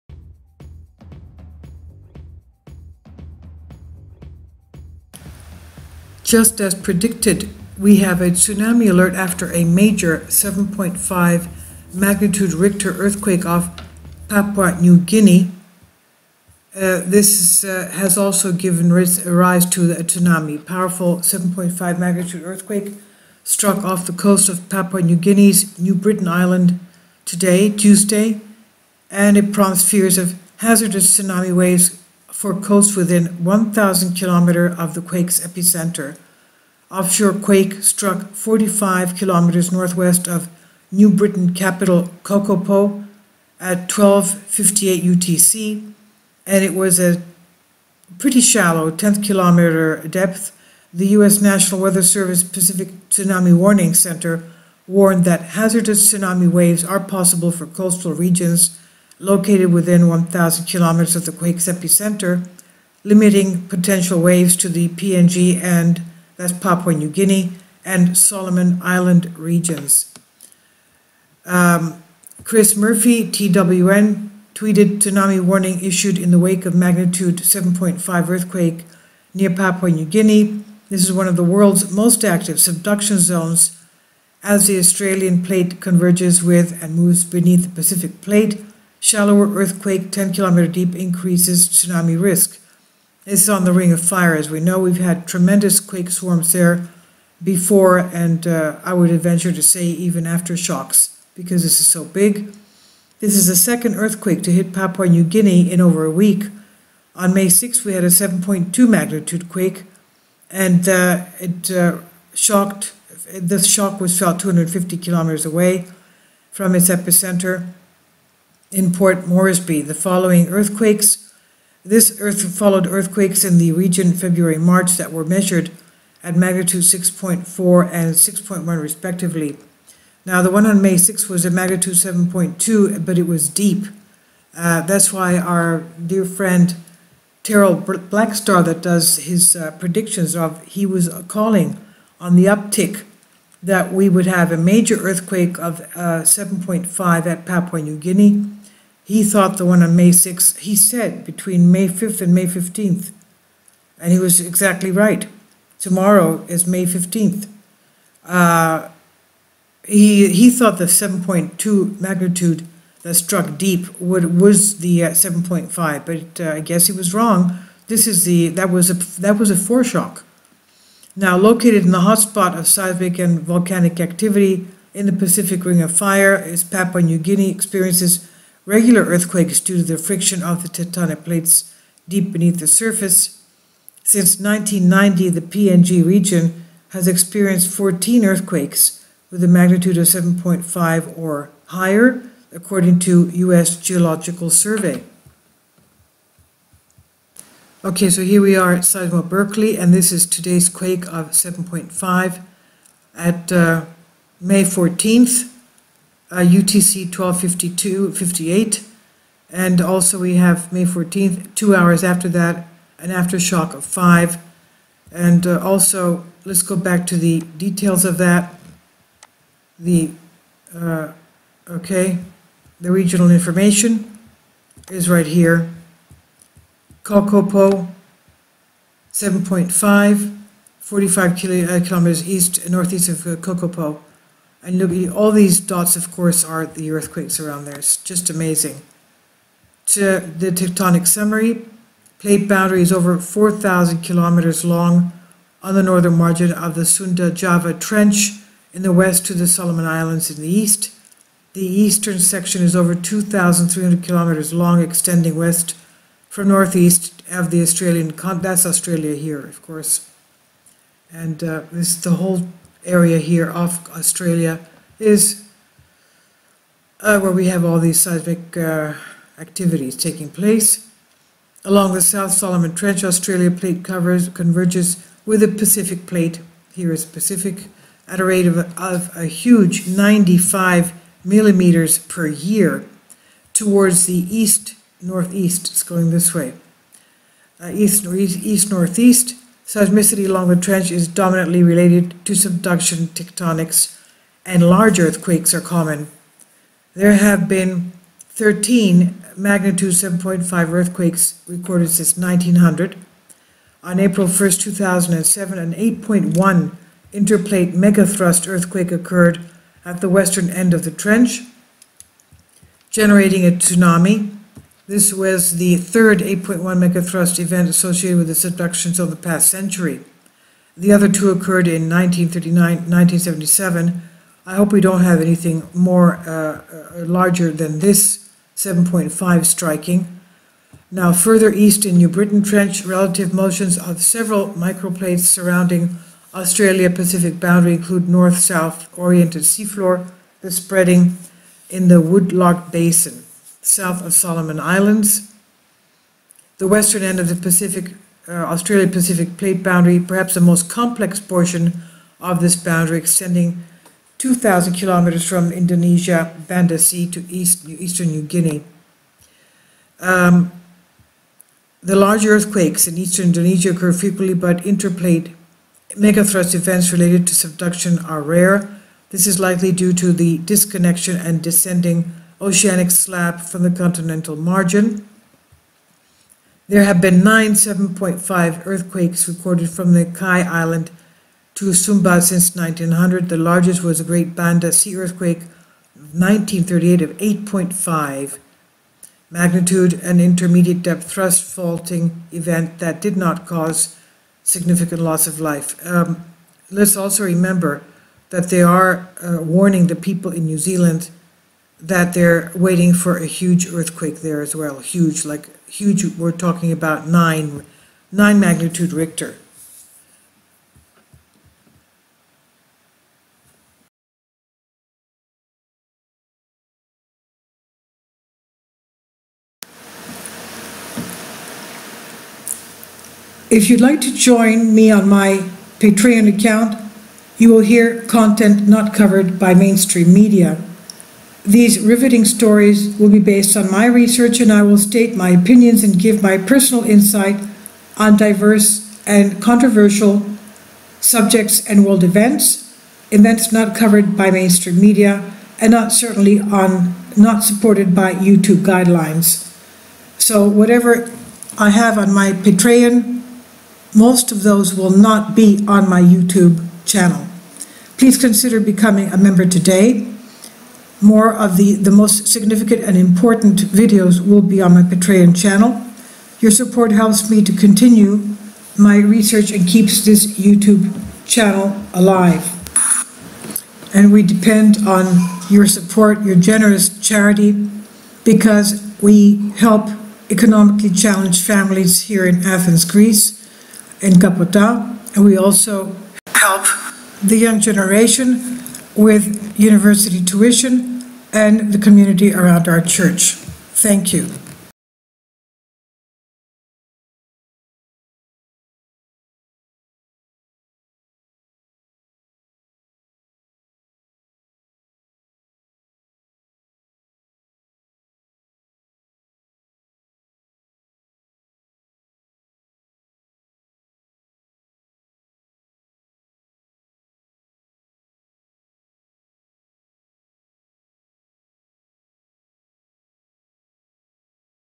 Tsunami Alert! After MAJOR M7.5 Earthquake off Papua New Guinea - USGS
Tsunami alert after 7.5-magnitude earthquake off Papua New Guinea - USGS